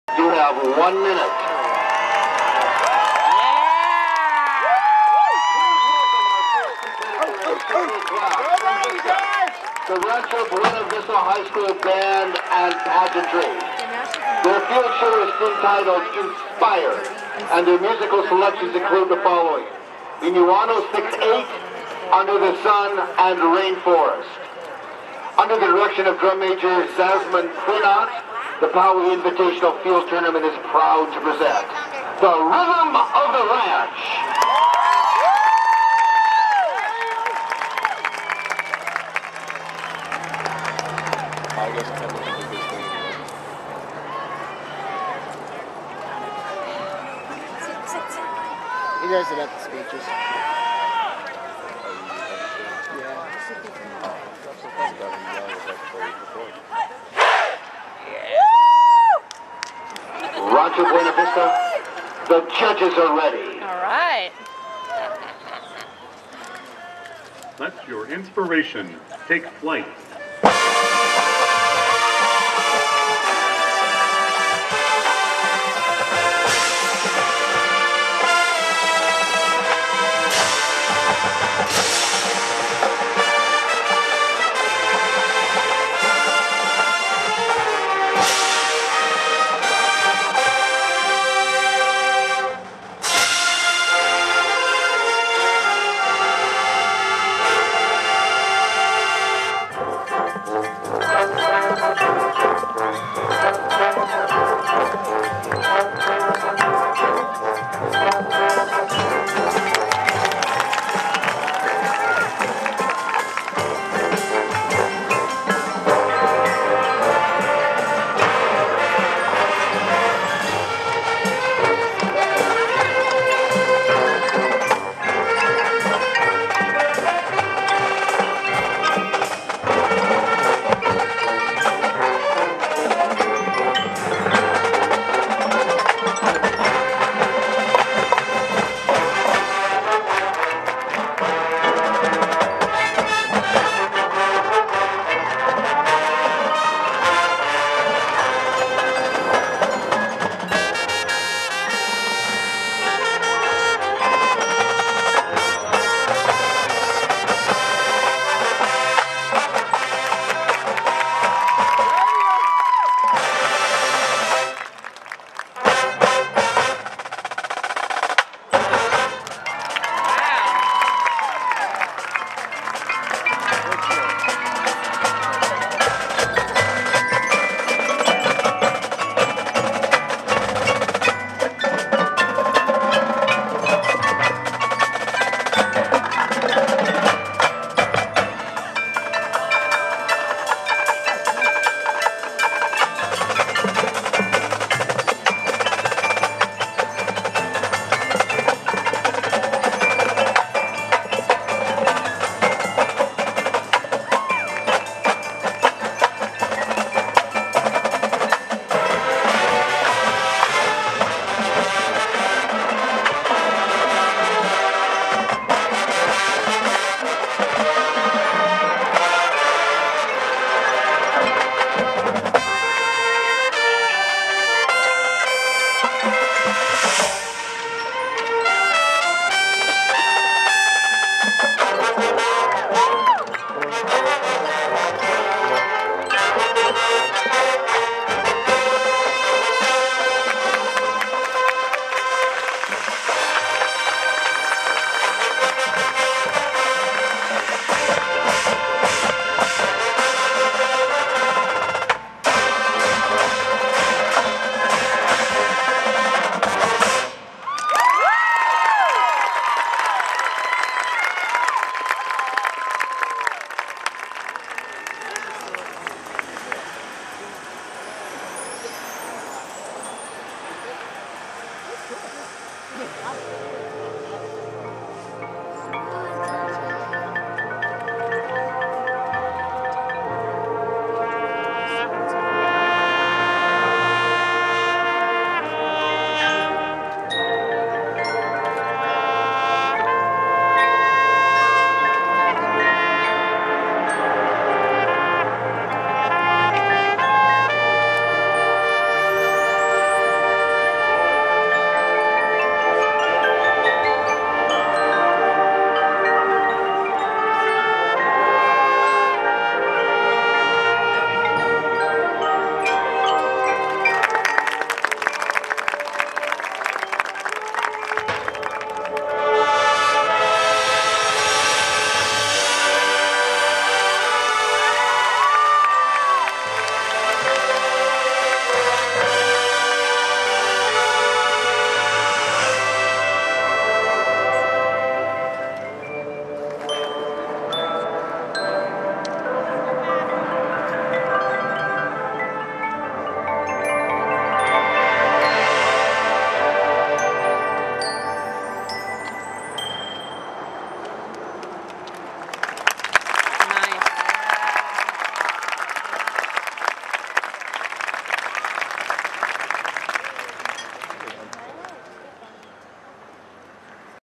Rancho Buena Vista HS (RBV) Band 2007 "Inspire" PART ONE
Hi, Everyone. I had to break the video file into 2 parts due to the high quality - file to large to upload in one chunk!  Sorry.  Look for Part 2. WAY TO GO RANCHO WE LOVE YOU! DENISE & JOEY.  Field Show at Poway High Oct. 13, 2007.  AWESOME!  No. 1 in my book!!